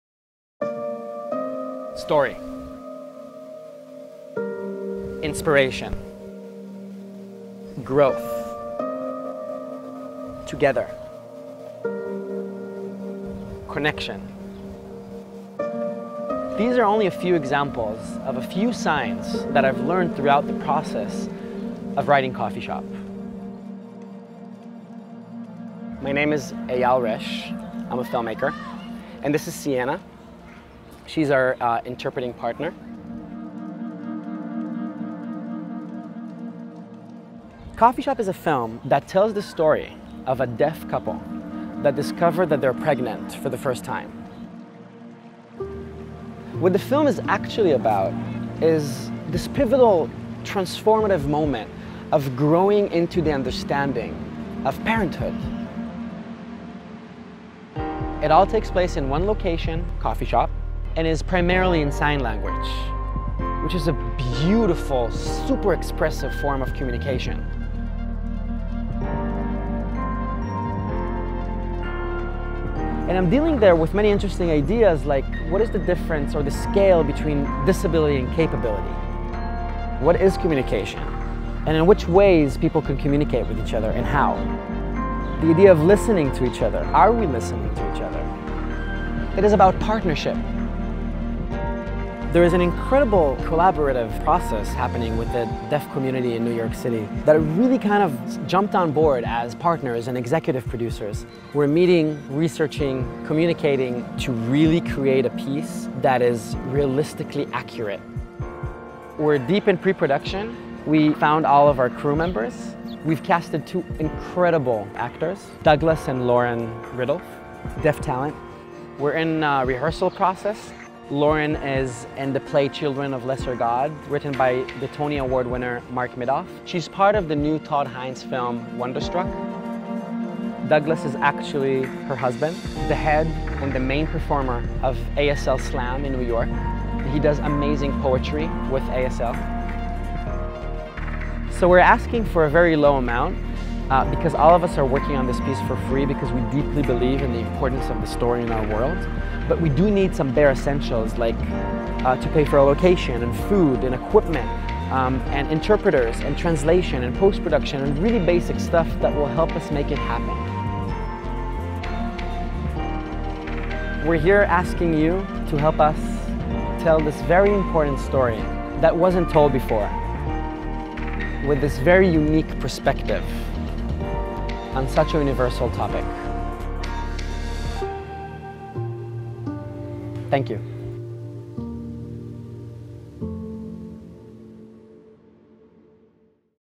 Indiegogo for Coffee Shop -- A Film by Eyal Resh
Our campaign video for the upcoming short film Coffee Shop (working title).
A Deaf couple discovers they are pregnant and their routine coffee shop thrums with everything they've left unsaid.